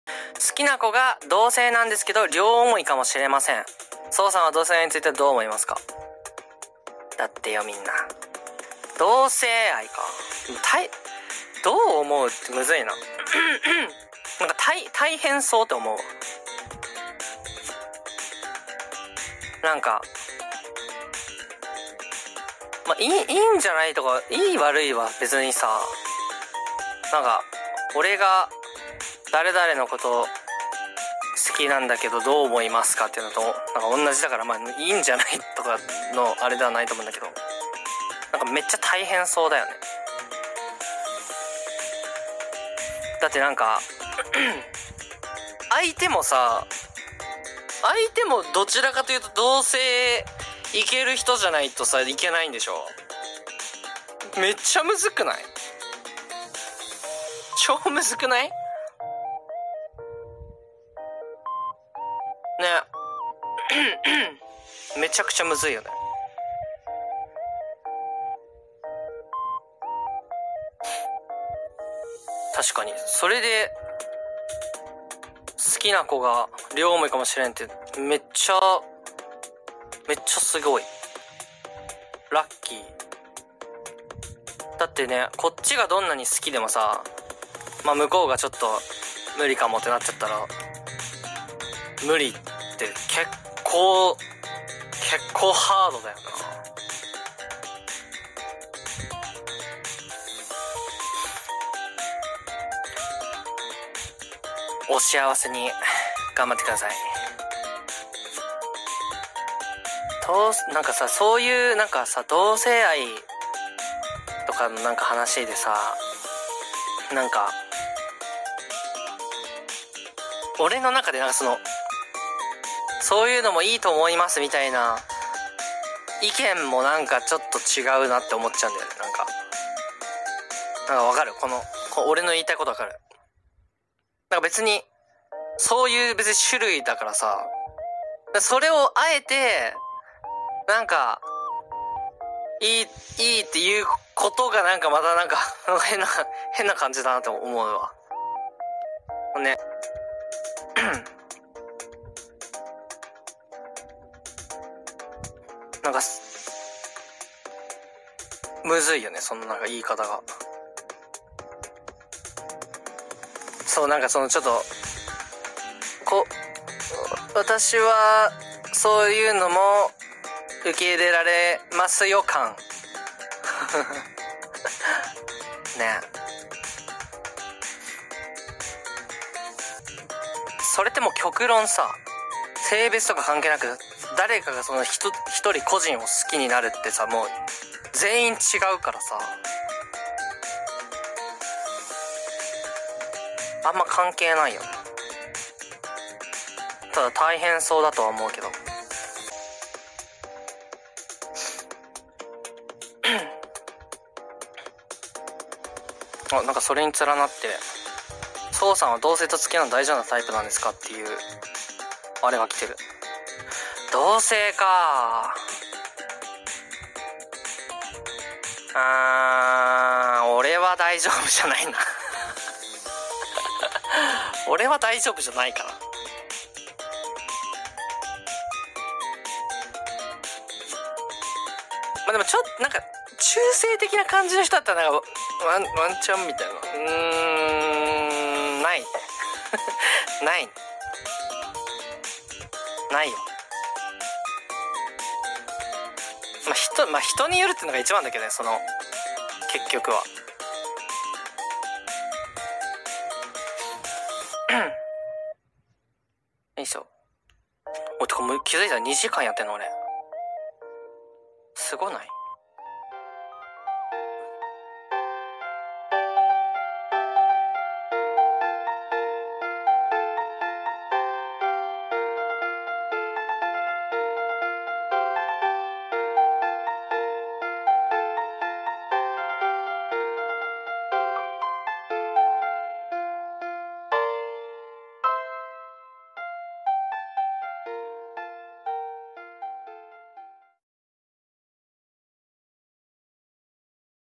[Sub Indo] Pendapat Sou Mengenai LGBT - Utaite Sub Indonesia
Kata kuncinya "Berusahalah untuk Bahagia"

Lakukan hal yang menurutmu benar dan membuatmu bahagia di Dunia dan Akhirat :)

Sc : Sou Twitcast

Maaf kalau ada kesalahan translate, kesalahannya bisa dibenarkan di kolom komentar